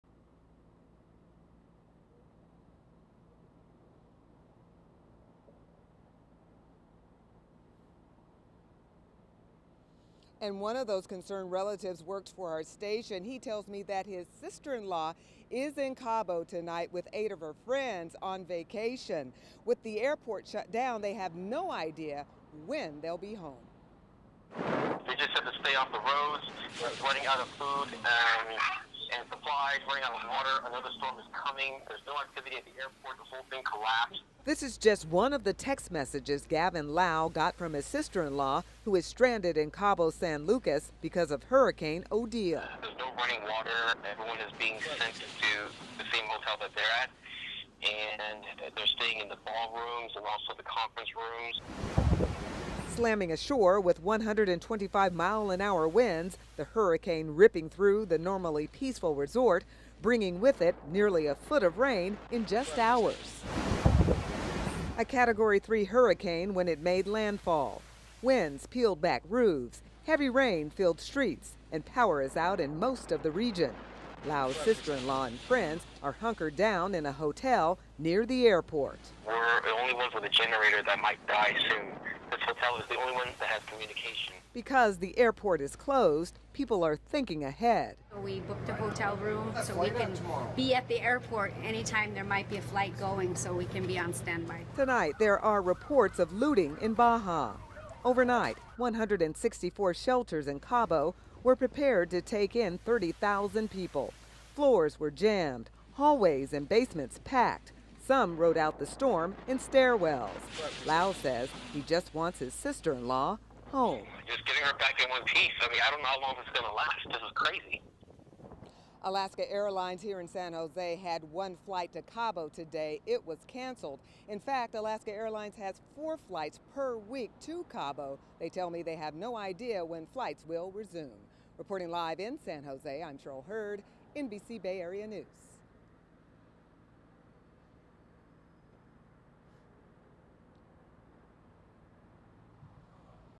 Bay Area Tourists Trapped In Cabo During Hurricane Odile 9-15-14
A quick get-away to Cabo is turning into a nightmare. A Category 3 hurricane ripped through Baja. The international airport in Cabo is now damaged and shut down. Major hotels have also been evacuated. Tourists and locals are now scrambling for safe shelter. NBC Bay Area’s Cheryl Hurd reports from San Jose airport where she spoke with worried relatives. I uploaded this primarily for the live shot intro and outro.